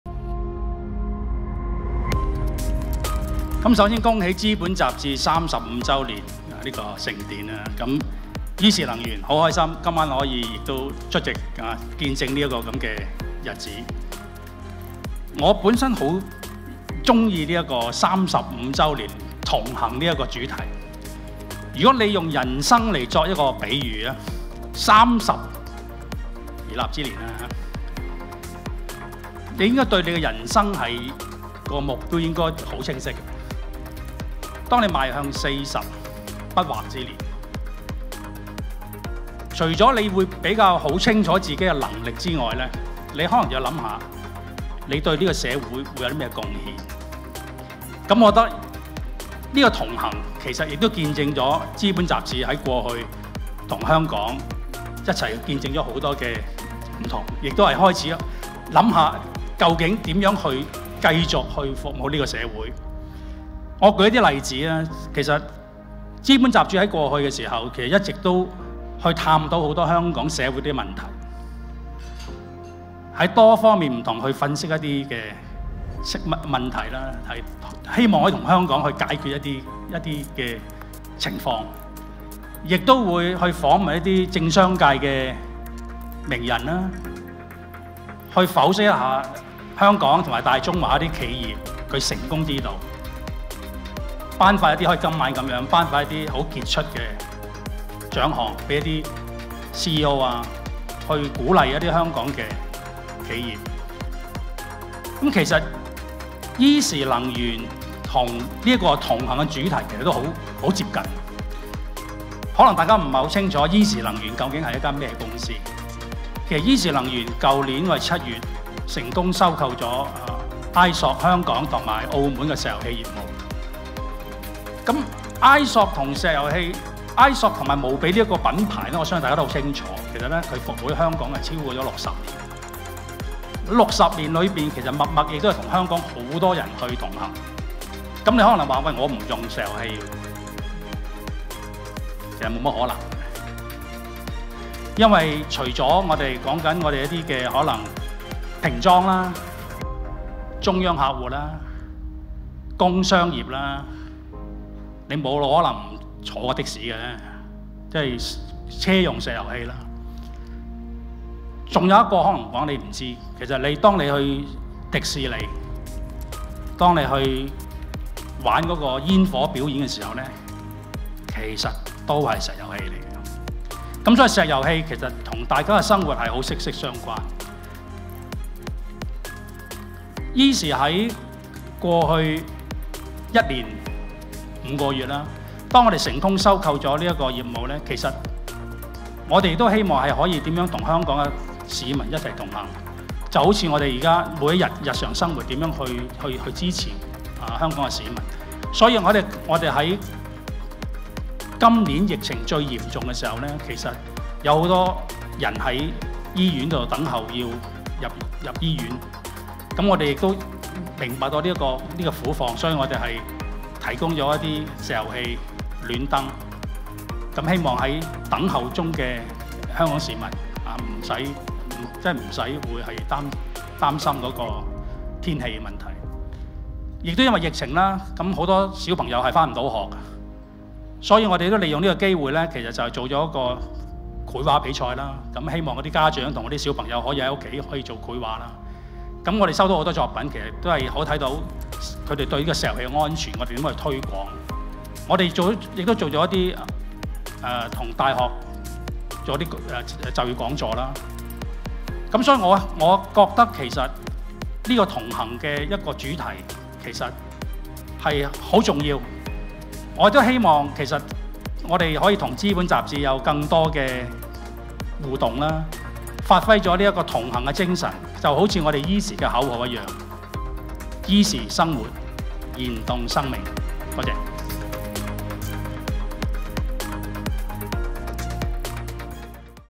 《資本雜誌》35週年晚宴暨 CEO x Entrepreneur Nite 2022 頒獎典禮 - 【同行獎】依時能源有限公司
【資本盛事】「《資本雜誌》35週年晚宴暨 CEO x Entrepreneur Nite 2022 頒獎典禮」

《資本雜誌》創刊35周年的晚宴上，冠蓋雲集，場面熱鬧，聚焦在港島香格里拉大酒店的一隅，象徵性地展示了香港工商界的無窮活力。全球經濟起伏不定，在挑戰重重的媒體市場上，《資本雜誌》能幸運地走過35年，是不容易的事，而在「不容易」的背後，更是憑著各界支持，彼此攜手，才能走到今日。

《資本》年度盛事，一如以往頒發個人獎項予業界精英。得獎者方面，便包括了獲頒「年度傑出企業家大獎」的卓珈控股集團有限公司主席兼行政總裁馬黎珈而女士；「年度傑出CEO大獎」則頒予晉羚財富管理集團創始人兼行政總裁李汶臻女士及中基長壽科學主席閆立先生。在這35週年的重要時刻，更特設「傑出貢獻大獎」，頒予亞洲聯合基建控股有限公司主席彭一庭先生及四洲集團董事總經理戴德豐先生。

立即Follow我哋，獲得更多精彩內容！